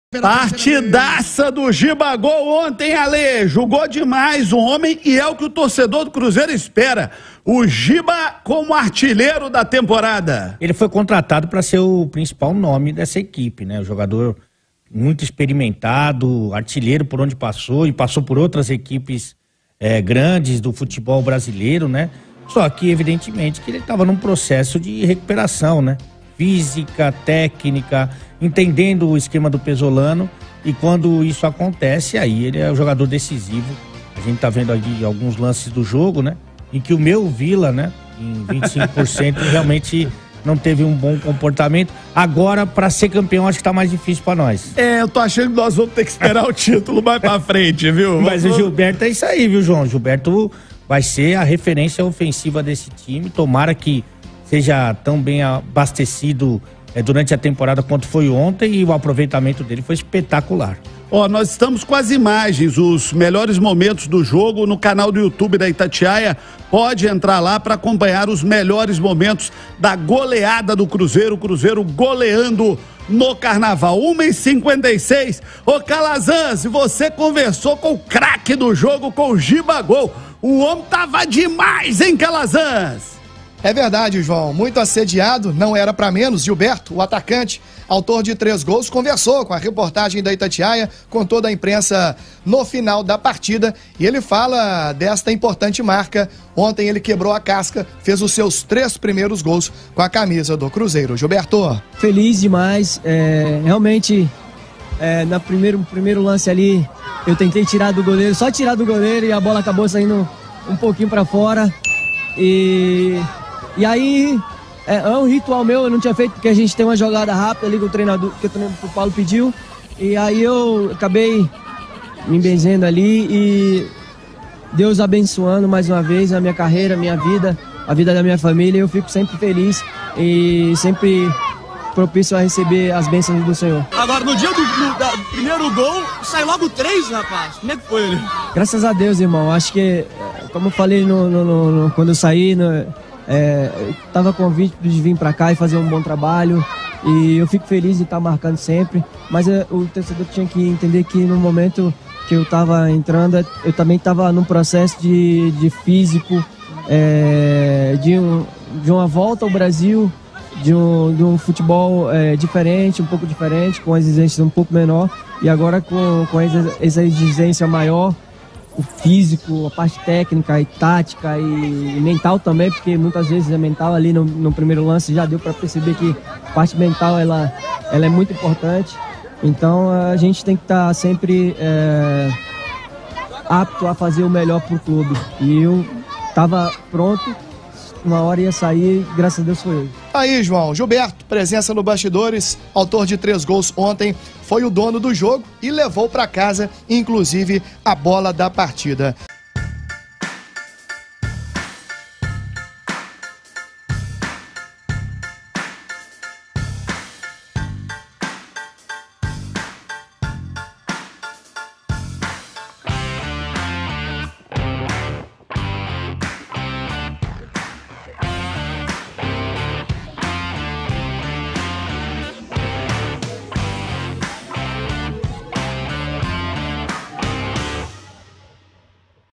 GILBERTO DESENCANTA E MARCA TRÊS GOLS CONTRA O VILLA NOVA; CONFIRA!
O atacante Gilberto balançou as redes pela primeira vez com a camisa do Cruzeiro, marcando um hat-trick logo de cara. Ao final da partida, o camisa 21 ressaltou a alegria de marcar com a camisa celeste e falou sobre ajudar a Raposa na sequência da temporada.

📲 Twitter.com/radioitatiaia
📸 Instagram.com/itatiaiaoficial
💻 Facebook.com/radioitatiaia